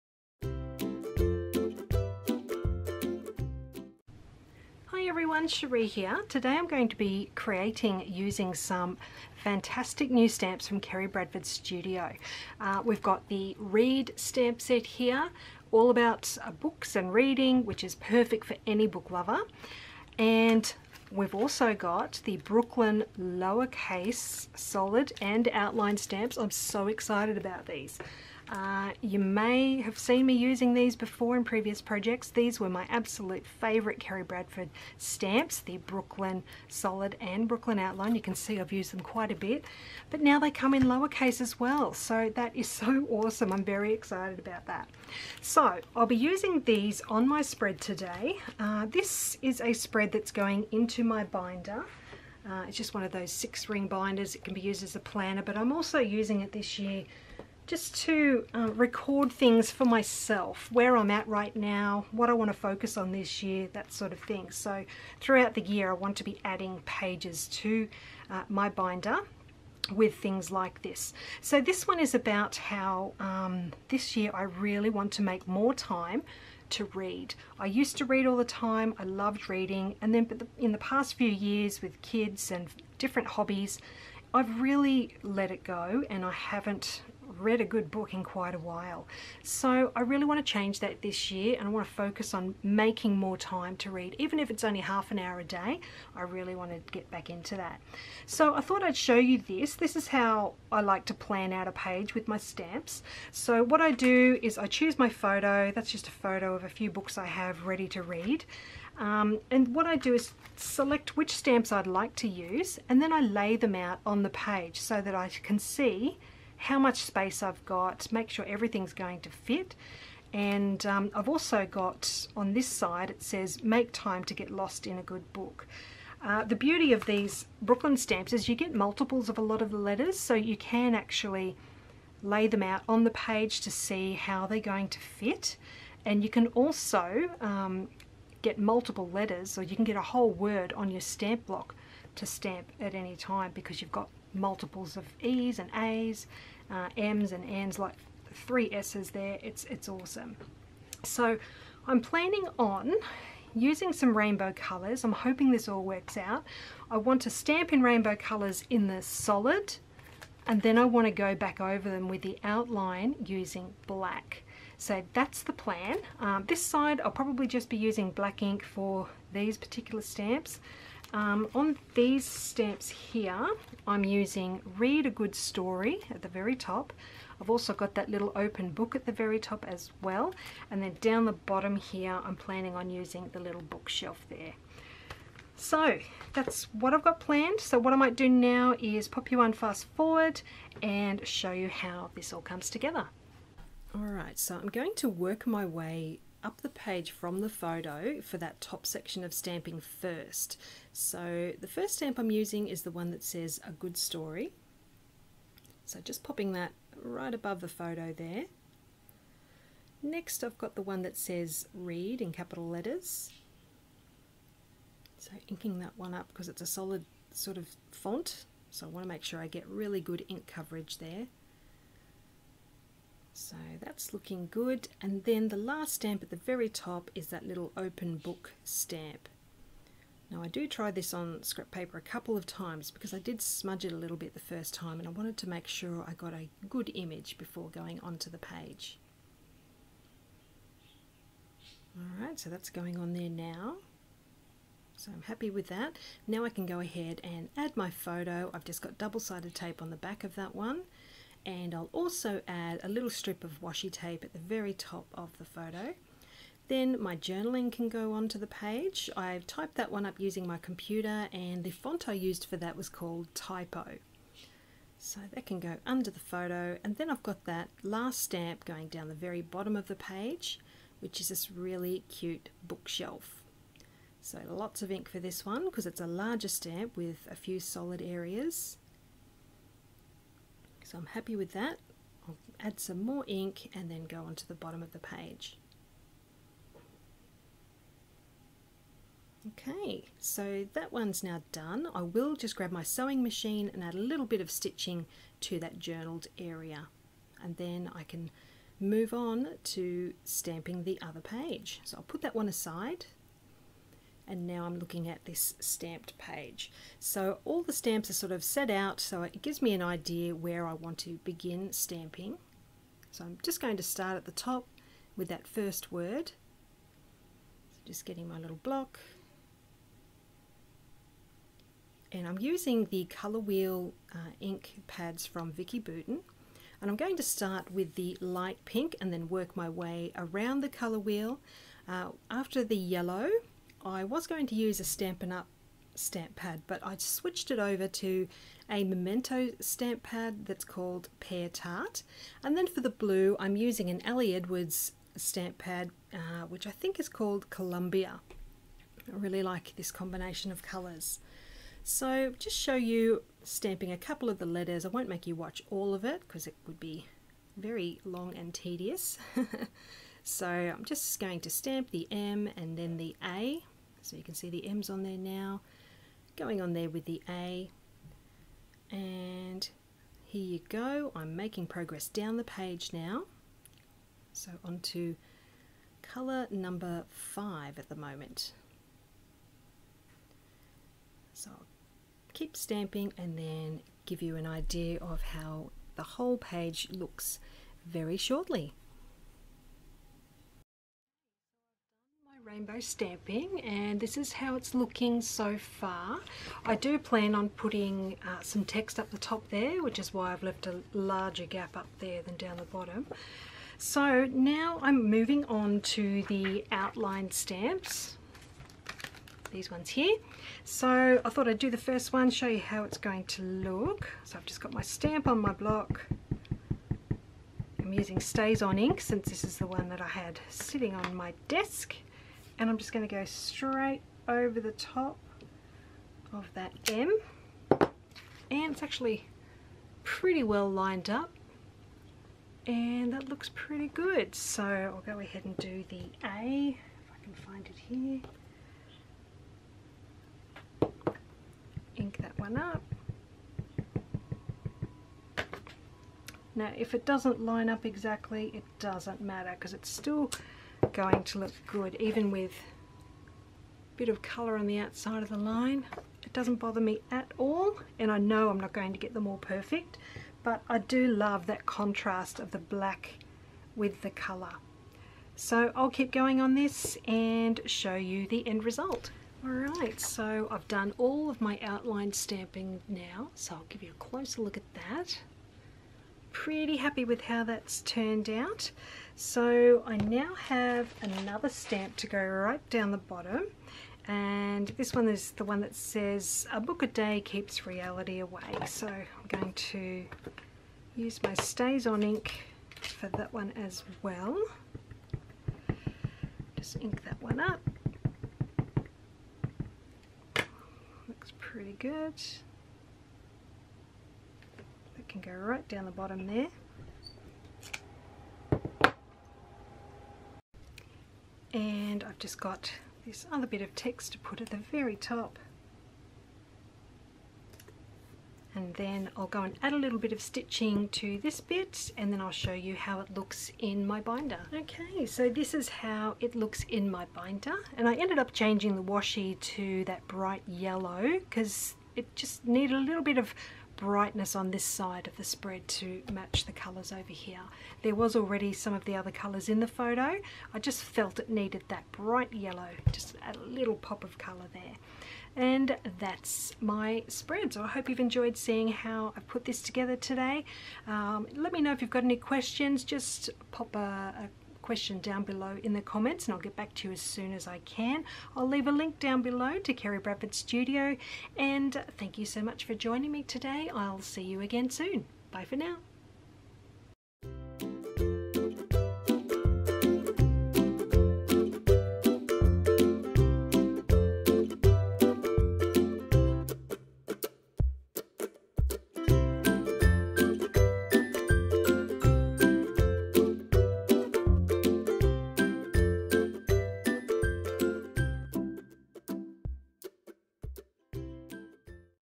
Scrapbooking Process | Read a Good Story feat. Kerri Bradford Studio Stamps
In this video I create a spread for my binder featuring the new  Read, Brooklyn Lower and Brooklyn Lower Outline stamp sets from Kerri Bradford Studio.

Please let me know if you have any questions.